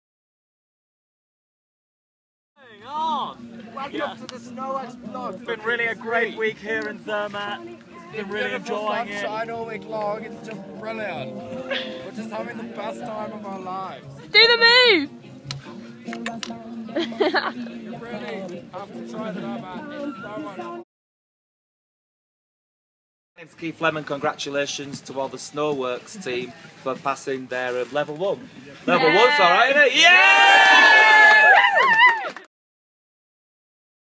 Week 3 GAP 2012 blog
Live feed from the team in Zermatt doing the BASI level 1 exam, with surprise guest appearance